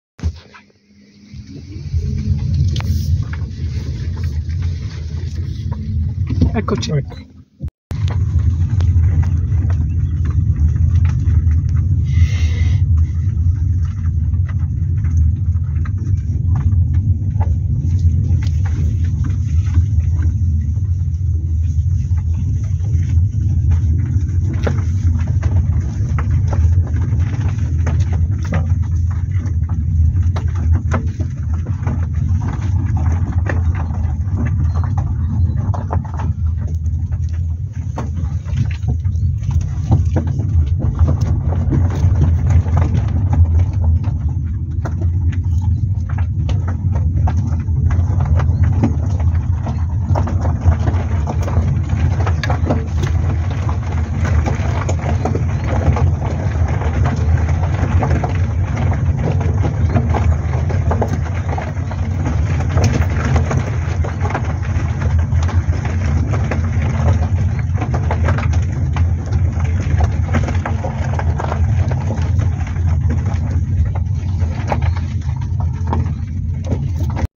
La strada che porta a Malga Slapeur, Altopiano di Asiago
Escursione al Monte Fior e  Città di roccia.
La strada che porta a Malga Slapeur, punto di inizio dell'escursione, è decisamente sterrata ma non presenta, al momento in cui l’abbiamo percorsa noi, buche o ostacoli che richiedano per forza di cose un mezzo fuoristrada.

Attenzione nei punti in cui non ci sono slarghi per far passare l’eventuale auto che provenga dalla parte opposta, come nel tratto che si incunea fra due pareti. "L'eccoci" che ci sentite dire nel video è proprio perché ci eravamo appena chiesti cosa avremmo fatto incontrando un'auto in direzione opposta. E niente, siamo tornati indietro fino al primo slargo. Il video prosegue con il percorso completo di questo tratto.